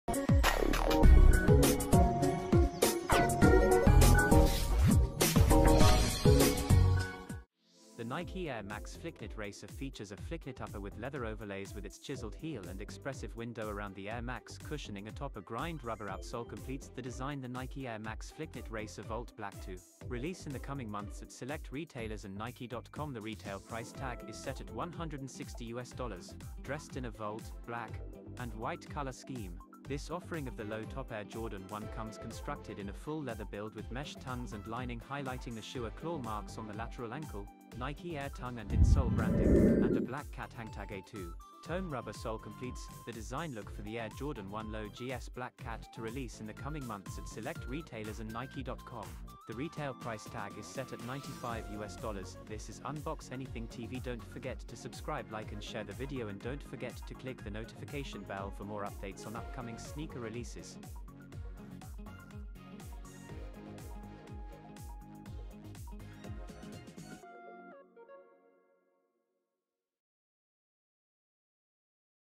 Nike Air Max Flyknit Racer"Volt/Black"/Air Jordan 1 Low SE GS"Black Cat”Detailed+Release date+Price
Air Jordan 1 Low SE GS "Black Cat'
Color: Volt/Black-White
Style Code: DX6666-701
Release Date: Summer 2023
Price: $95

Nike Air Max Flyknit Racer
Color: Volt/Black-White
Style Code: FD4610-700
Release Date: 2023
Price: $160